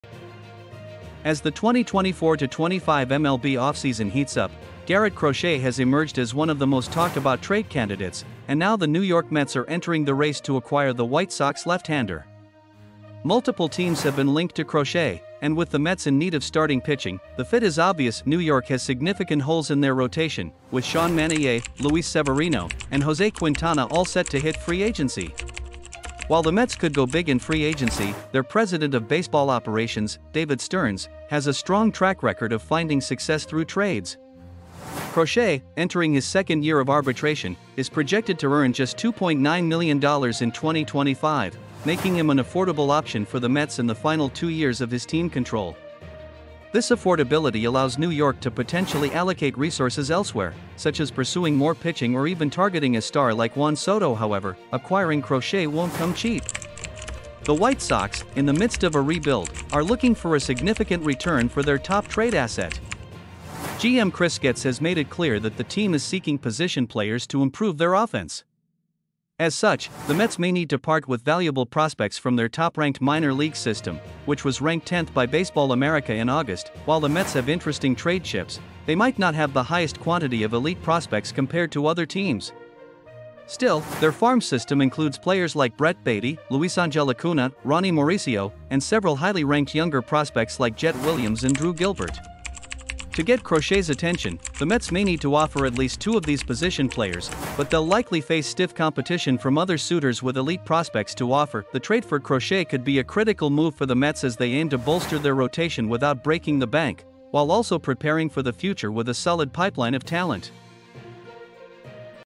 Mets Eyeing Garrett Crochet for Pitching Boost What They’re Willing to Offer!
The New York Mets are reportedly interested in acquiring talented left-handed pitcher Garrett Crochet from the Chicago White Sox. In this video, we dive into the potential trade, analyzing what the Mets might offer to land Crochet and how he could impact their pitching staff. Join us as we break down the latest rumors and discuss the potential implications for both teams.